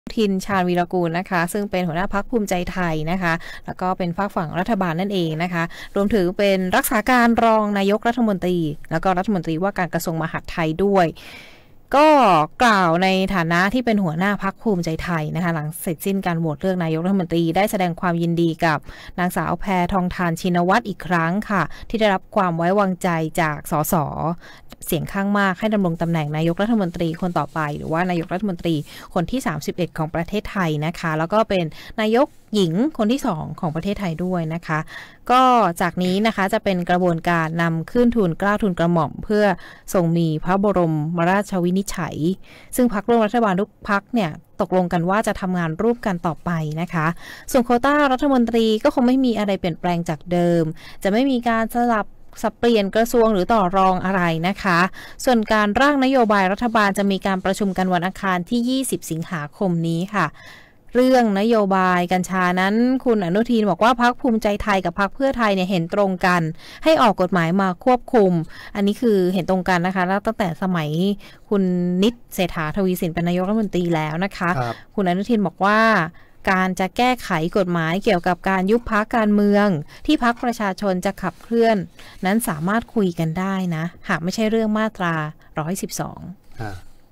รายการข่าวยามเช้า อนุทิน ยินดีกับว่าที่นายกฯ คนที่ 31 หลังได้รับความไว้วางใจจากสมาชิกสภาฯ 319 เสียง
รายการข่าวยามเช้า อนุทิน ยินดีกับว่าที่นายกฯ คนที่ 31 หลังได้รับความไว้วางใจจากสมาชิกสภาผู้แทนราษฎร 319 เสียง มั่นใจ โควตารัฐมนตรีไม่มีอะไรเปลี่ยน